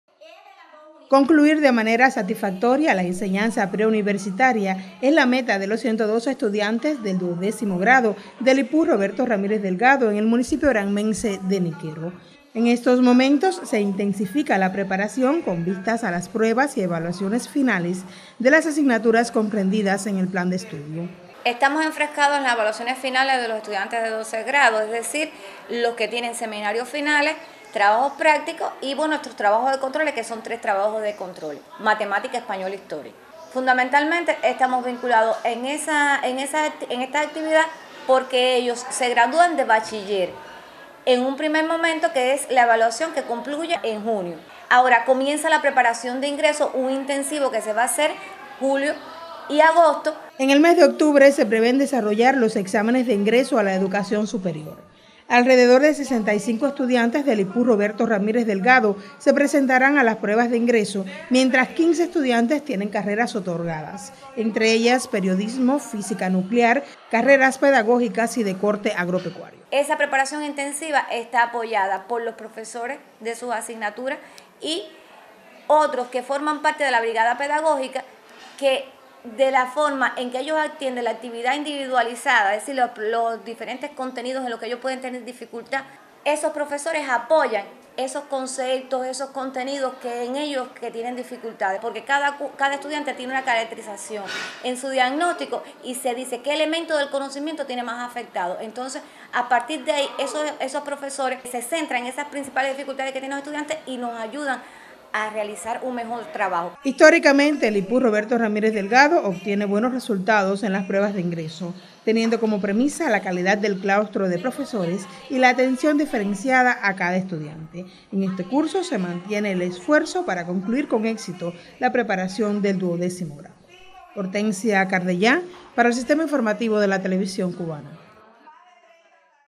Intensifican preparación de estudiantes del 12 grado con vistas a las pruebas de ingreso en Niquero.
Profesores del duodécimo grado en el municipio de Niquero intensifican la preparación de los alumnos con vistas a las evaluaciones finales para obtener el título de bachiller. Una vez concluyan los exámenes, comienza la preparación para las pruebas de ingreso a la educación superior en ese territorio del sur granmense.